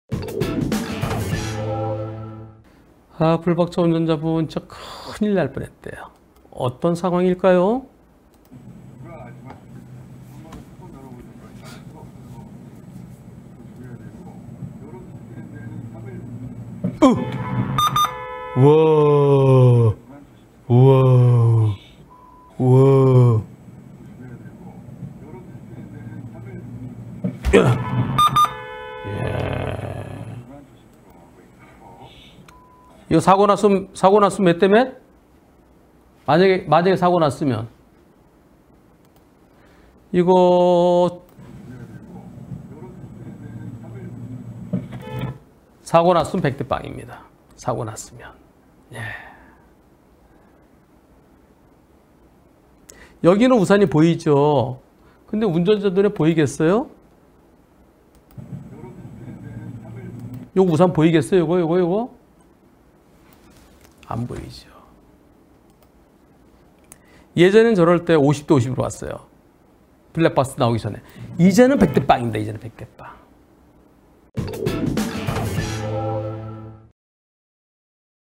21646회. 우산 들고 무작정 뛰고 보는 무단횡단자
65674
240321 (목) 생방송 1부 #10
정말 식겁한 순간!

1차로로 가고 있는 블박차 앞으로 왼쪽 반대편 차들 사이에서 우산 쓴 사람이 뛰어나와 사고 날 뻔한 상황.

연희로 무단 횡단하는 보행자와 사고 날 뻔했습니다.

* 의견 *

- 만약 사고가 났어도 무단횡단자 100% 과실인 상황입니다.
- 운전자 눈에는 우산이 보이지 않았을 가능성이 있습니다.
- 블랙박스 나오기 전에는 50:50으로 봤습니다. 하지만, 지금은 무단횡단자 100%입니다.